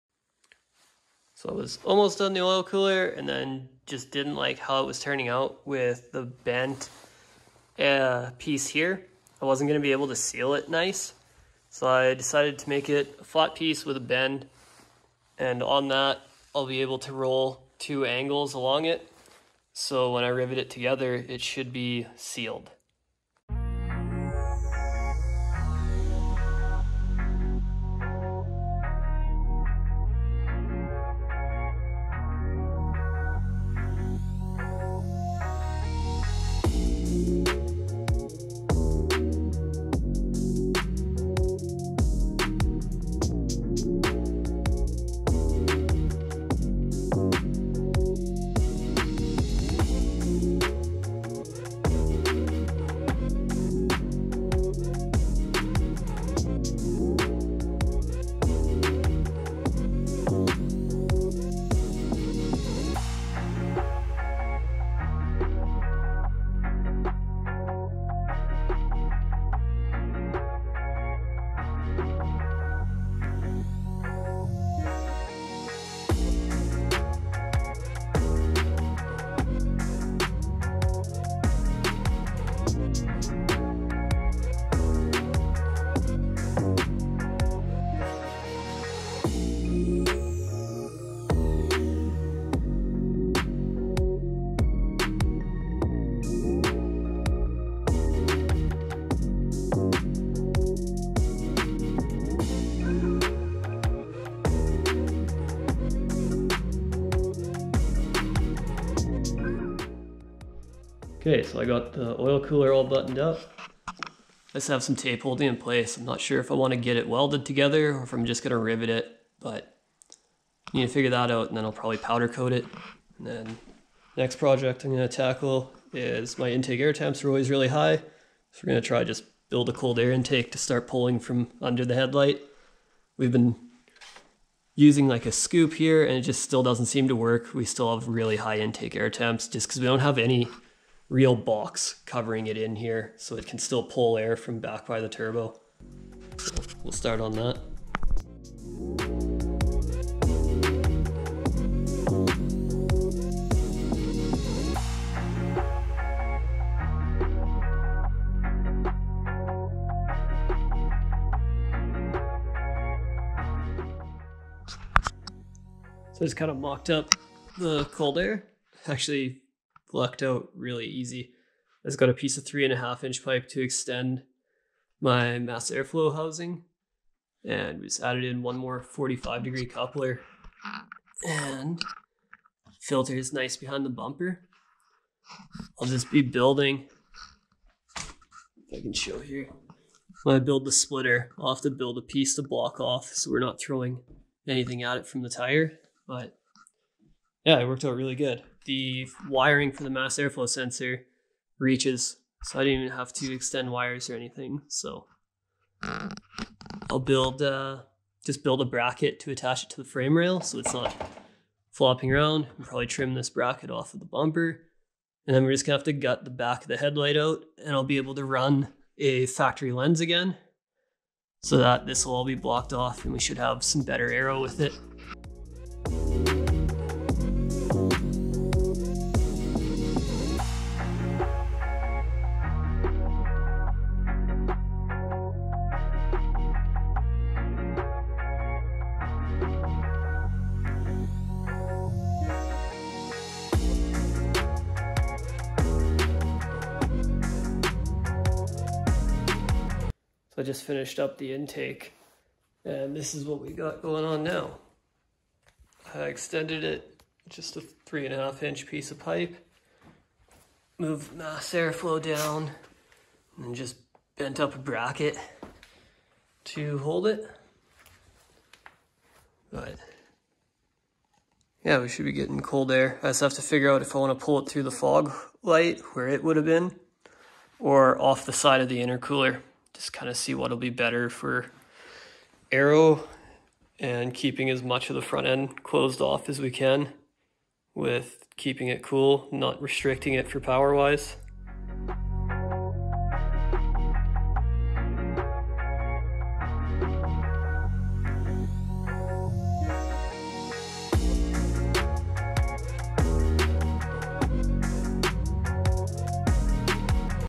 Evo X Oil Cooler Ducting and Cold Air intake
Building ducting to my oil cooler and modifying my intake